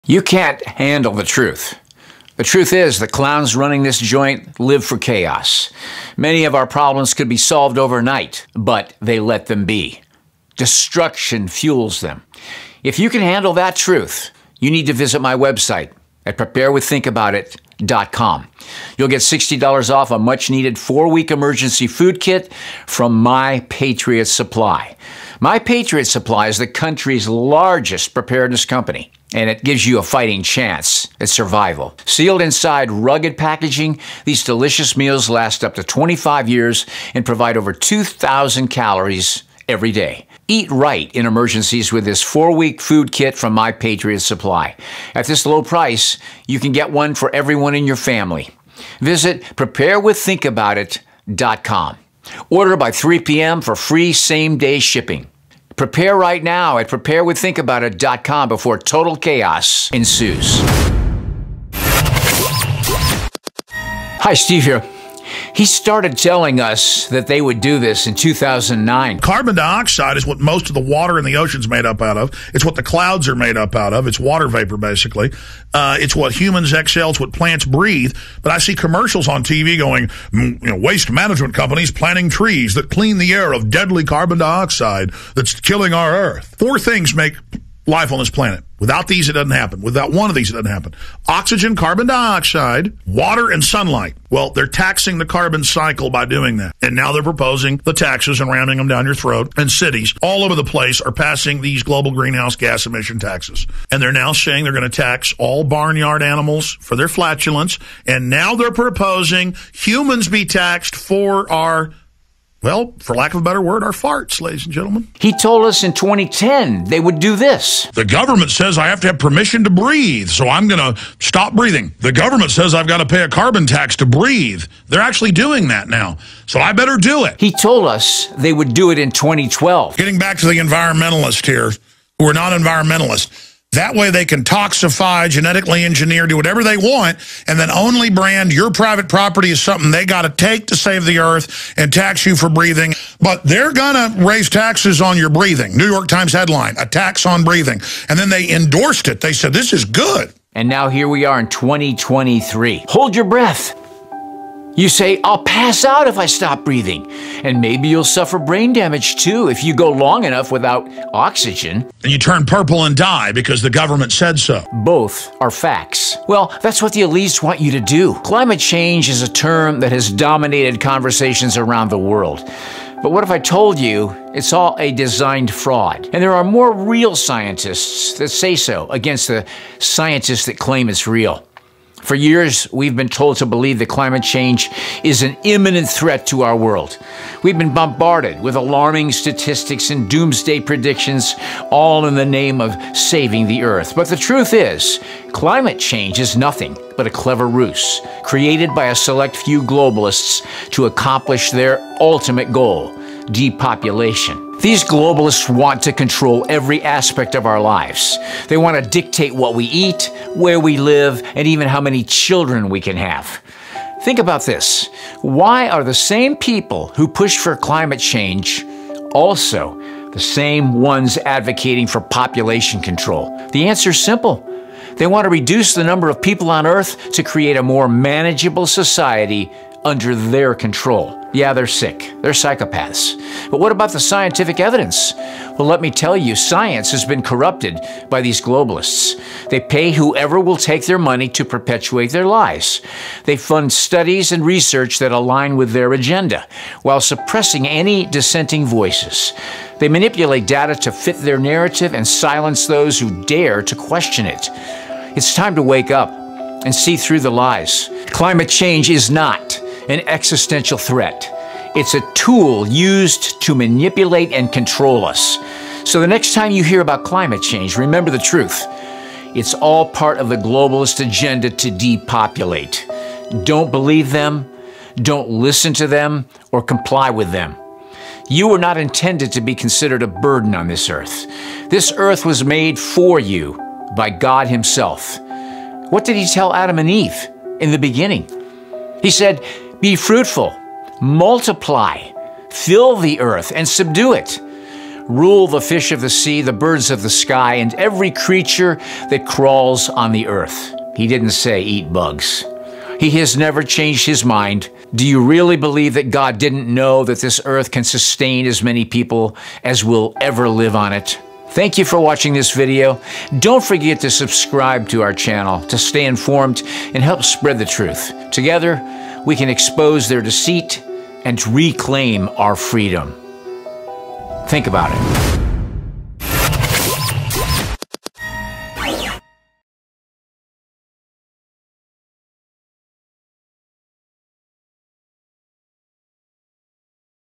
CLIMATE CHANGE
✅ $60 OFF their best-selling 4 Week Emergency Food Kit when you go to my special web

Climate change is a term that has dominated conversations around the world. But what if I told you it's all a designed...

If you would you like to pray to accept Jesus Christ as your Lord and Savior... Click this